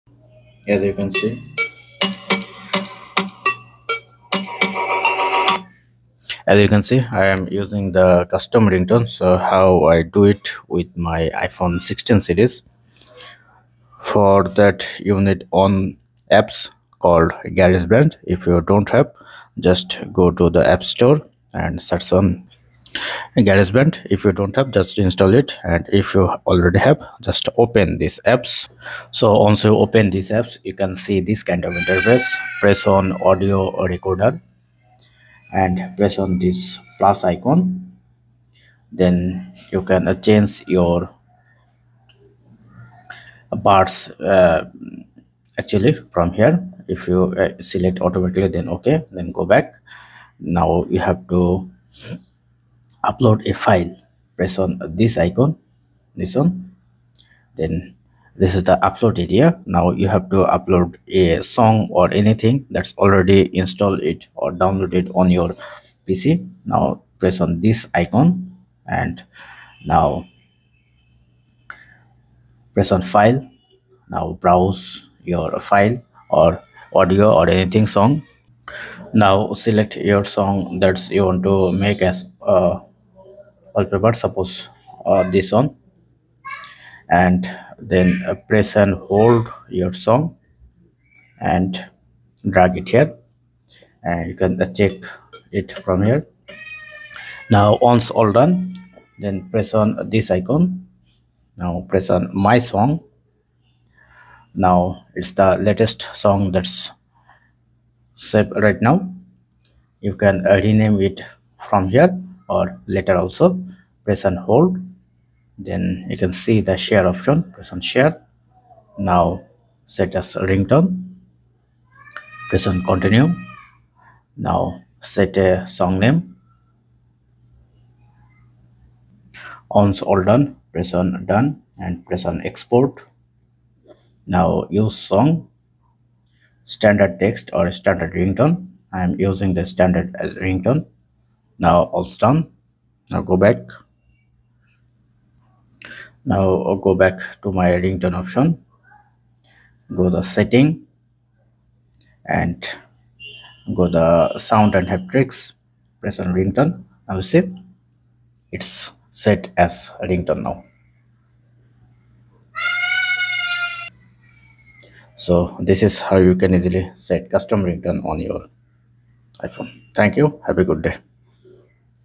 How to set custom ringtone on iPhone 16 pro/16 ProMax
Here are the easy steps on personalising your device's sound and how to set up custom ringtones on an iPhone. Read more on Croma Unboxed.

How do I add custom ringtones to my iPhone?
Can I set my own song as a ringtone on iPhone?
How to set a custom ringtone in iPhone without iTunes?